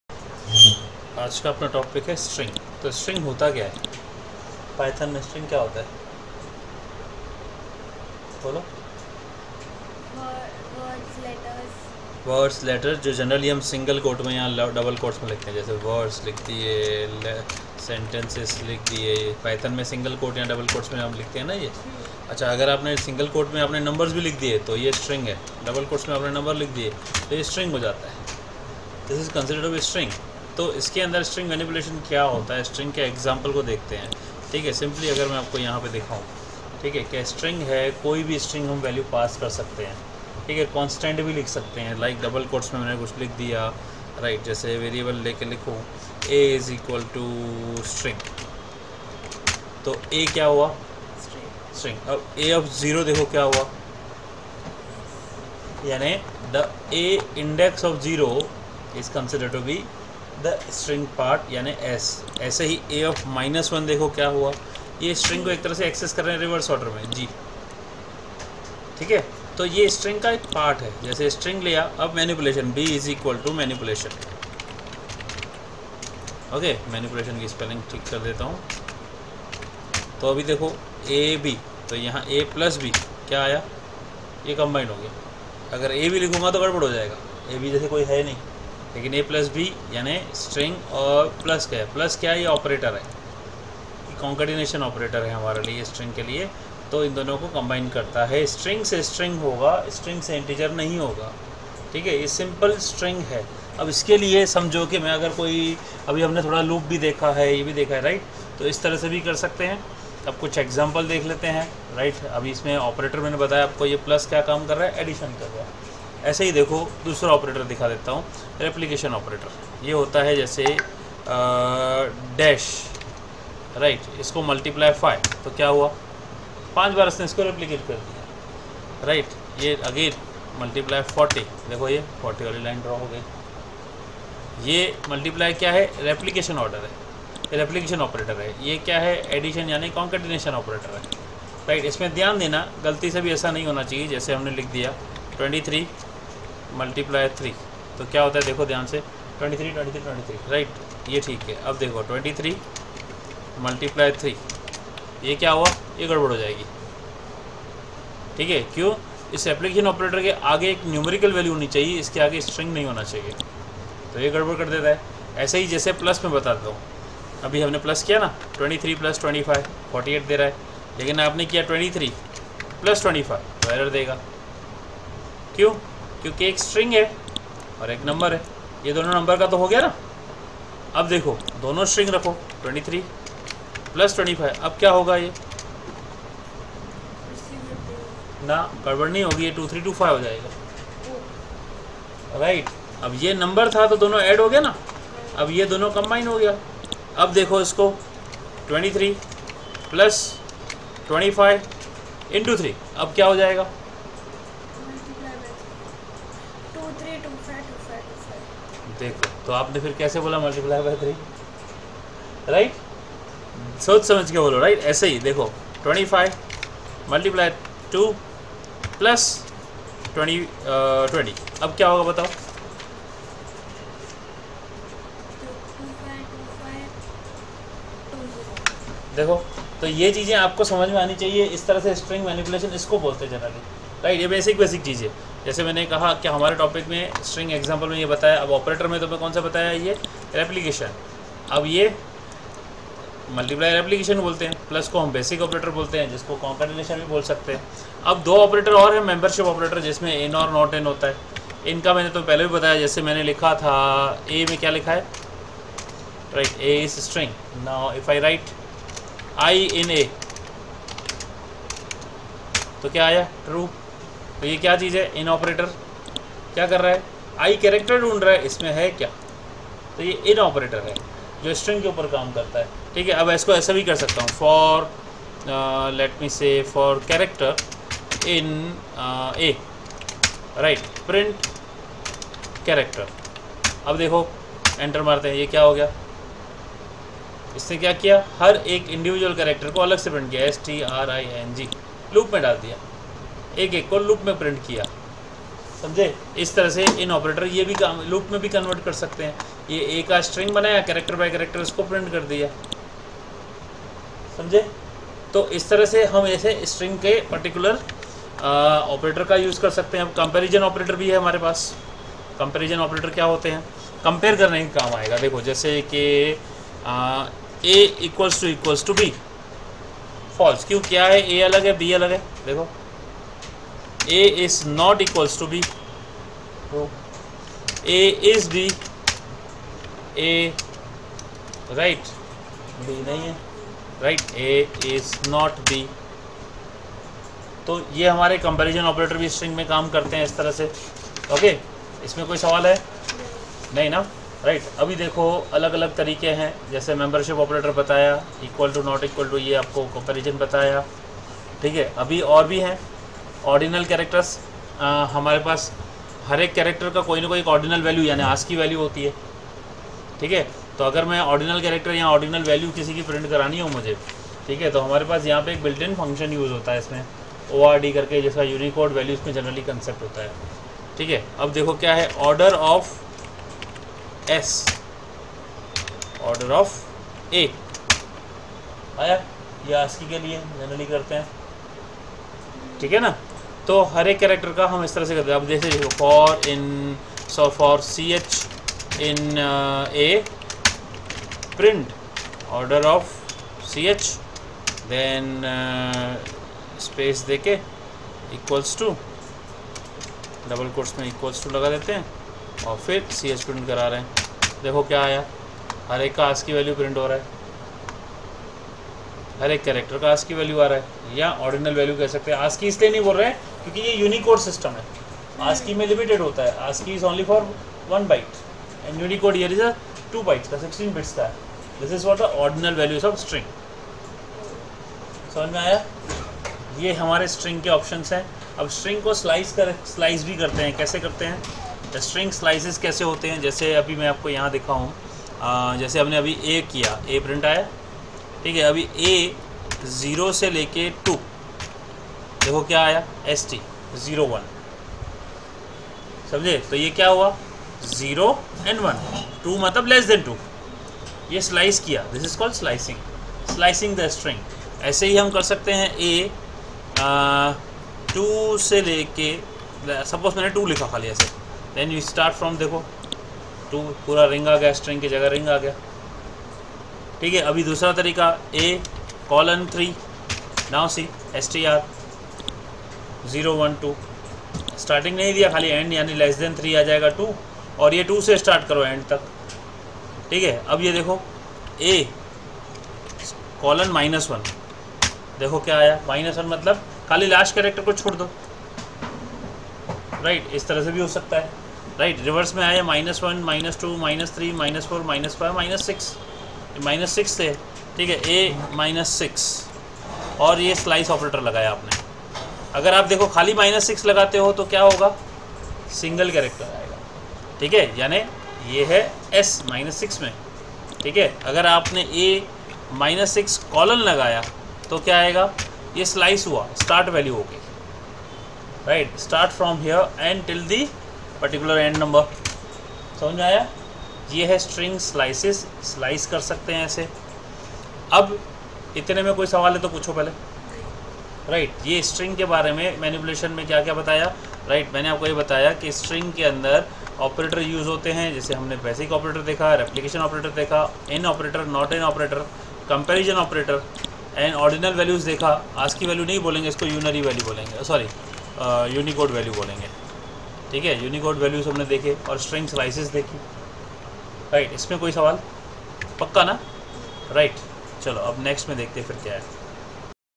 Python | String Manipulation
String Manipulation
String Examples

String Operators
 - Basic Operator        +
 - Replication operator  *
 - Membership Operator   in,  not in
 - Comparison Operators ==, !=, is,  is not

Characters and their ordinal values (Unicode values)
String Slices

A Channel for the Computer Learners...